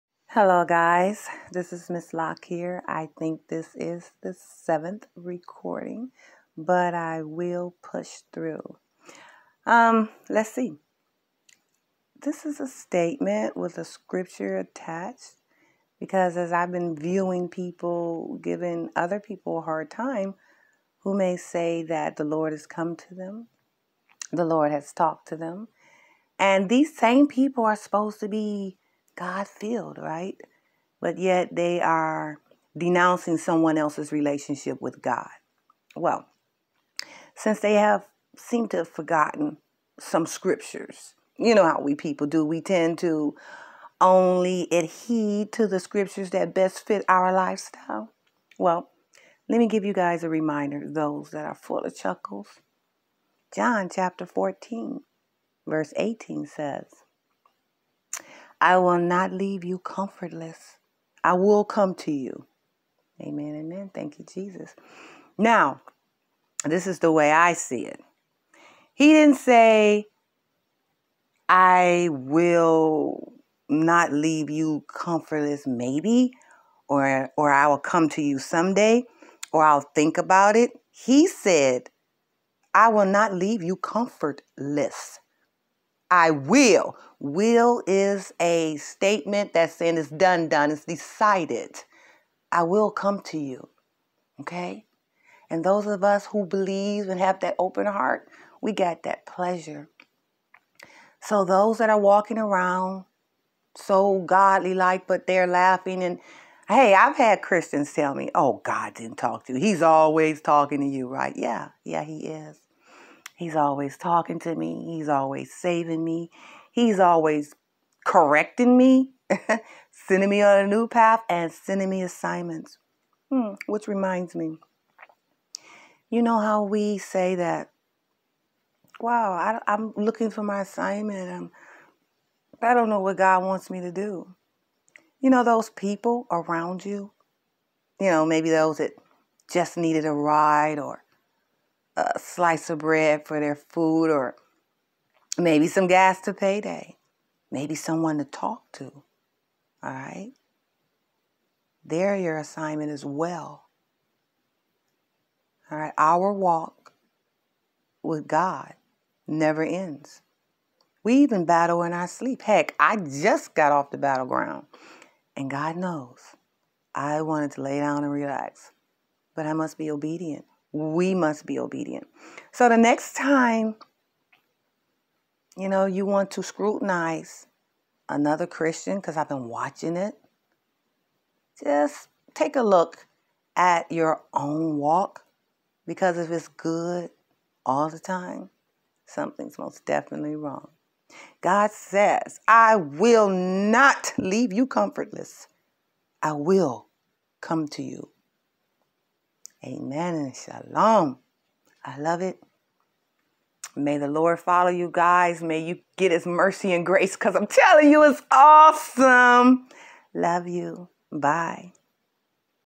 Why are you upset when they say God spoke to them? yet you claim to know the word.
God came to me they say and your reaction 😒🧐🤯